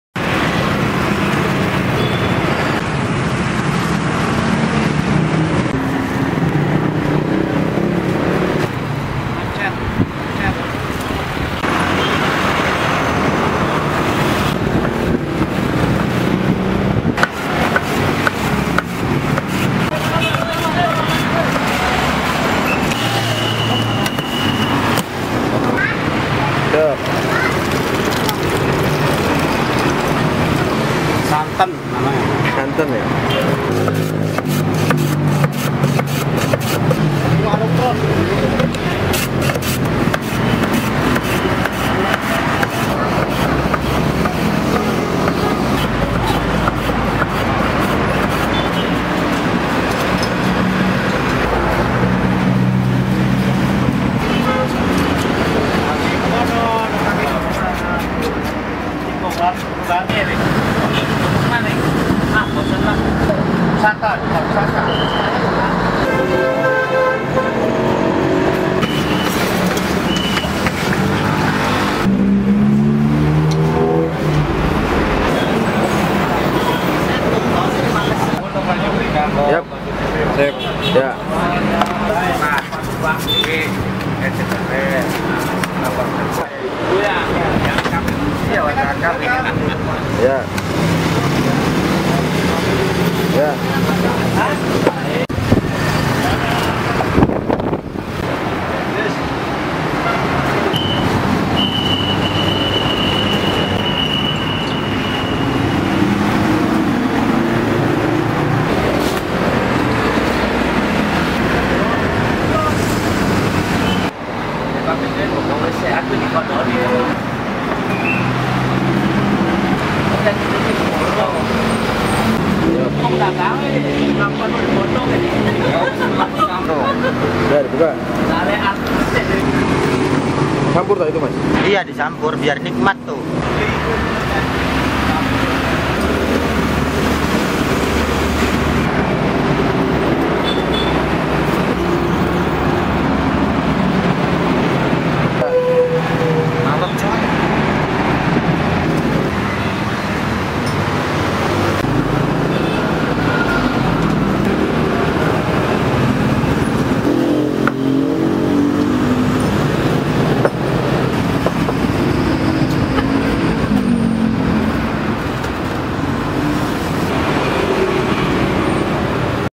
Nikmati Sensasi Segar Es Serut Pantura Cirebon
keberadaan es serut di jalur pantura diklaim mampu menghilangkan rasa dahaga saat berada di tengah teriknya jalur pantura.